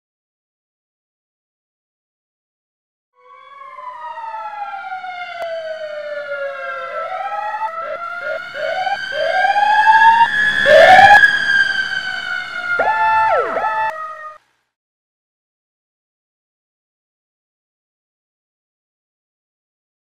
ENFD E12 responding
At 12:00 hours ENFD was alerted to respond to the dollar tree for a fire alarm.

E12 arrived O/S and reset the alarm
All units returned in 11 Minutes.

E12 is a 2020 Peirce Enforcer pumper